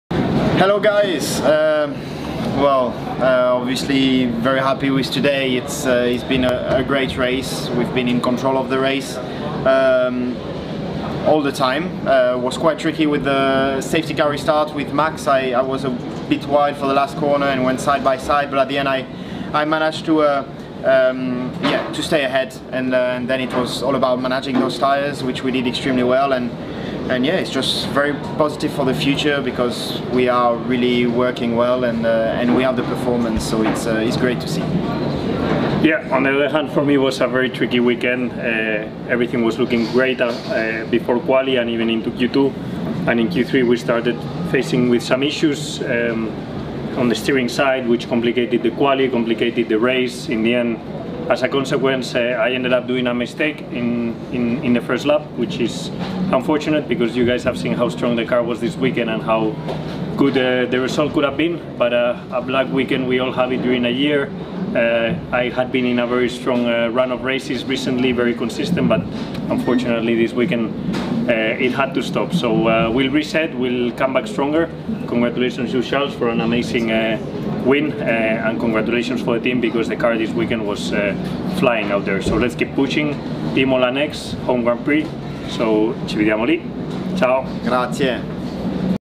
F1 AusGP Ferrari Drivers message
Ferrari drivers Charles Leclerc vs Carlos Sainz message after Australia GP.
F1 izle📺yarış sonucu🔥puan durumu.
Watch F1🏎race results🏁standings.